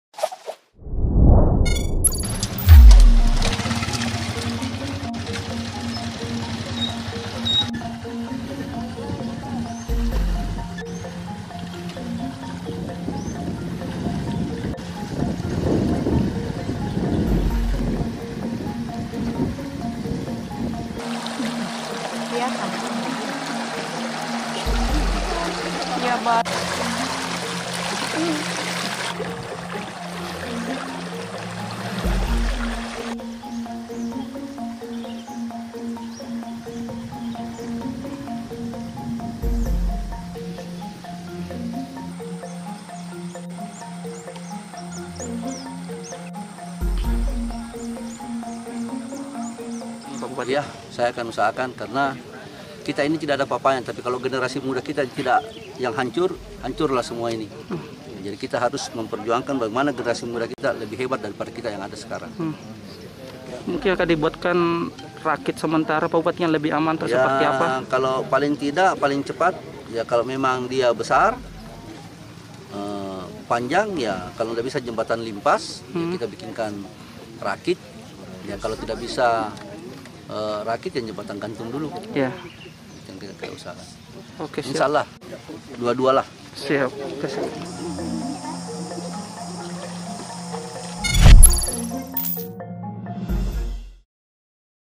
Pelajar di Polman Bertaruh Nyawa Demi Sekolah, Bupati Janjikan Ini
Pelajar di Polewali Mandar (Polman), Sulawesi Barat (Sulbar), harus bertaruh nyawa demi bisa bersekolah karena menyeberangi sungai dengan arus yang deras. Bupati Polman, Andi Ibrahim Masdar pun berjanji akan memberi bantuan, tapi tahun 2022.